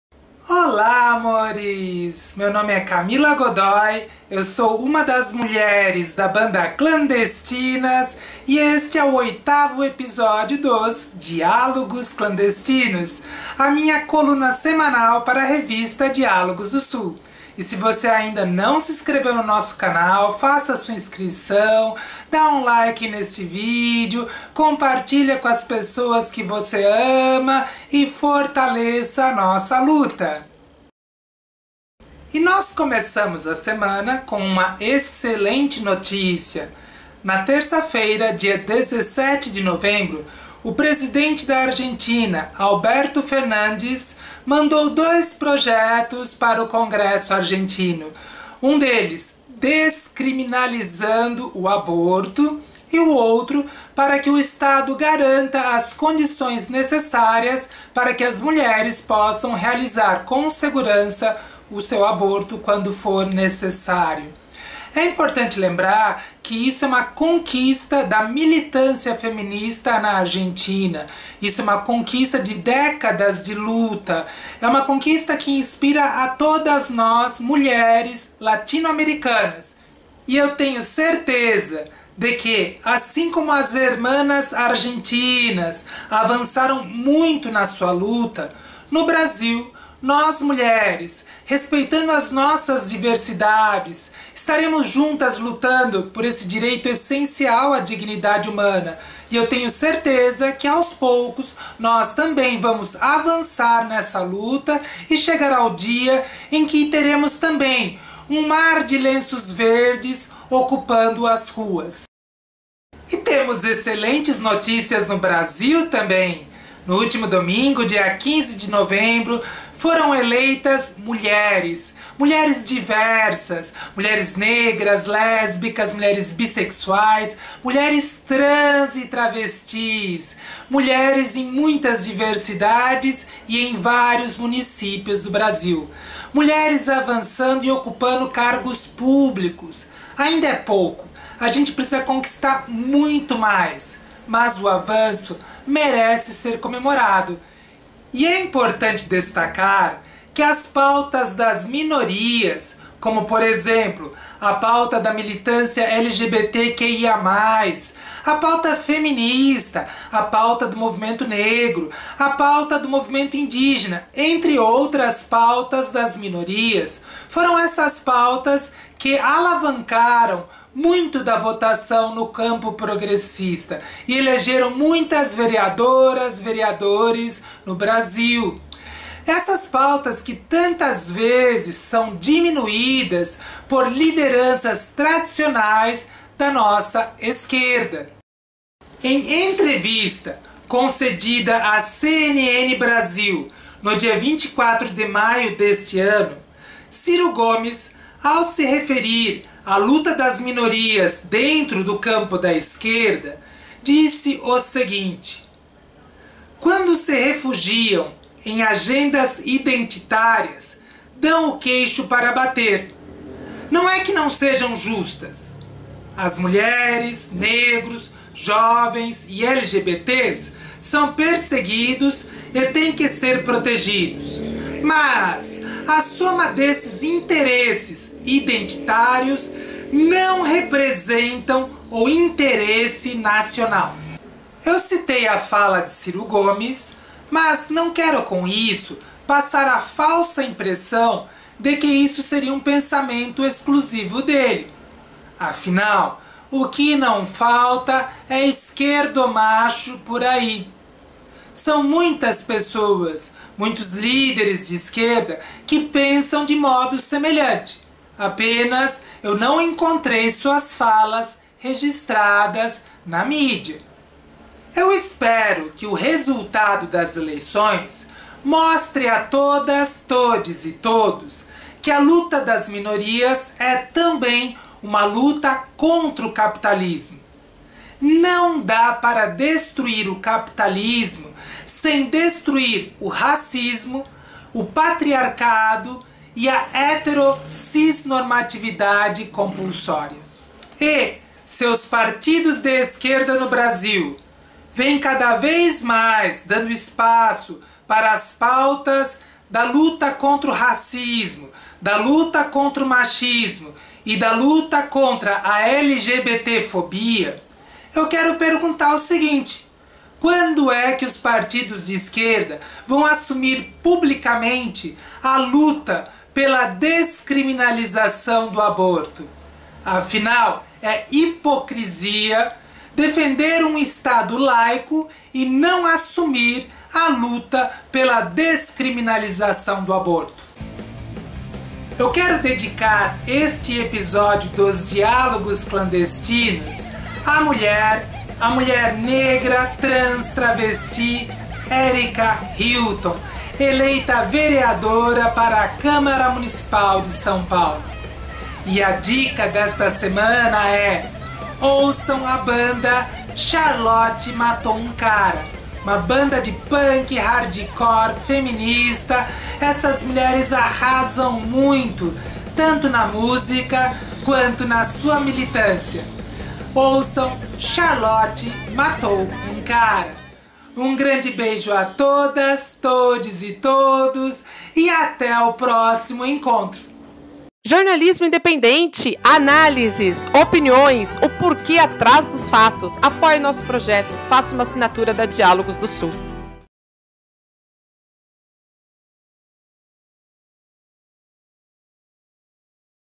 A luta das minorias e a revitalização das esquerdas - Diálogos Clandestinos #8
A musicista, militante feminista e baixista da banda clandestinas Camila Godoi aborda a luta das minorias simbolizada nas eleições municipais e a renovação vinda das urnas que alçou diversas novas mulheres à política.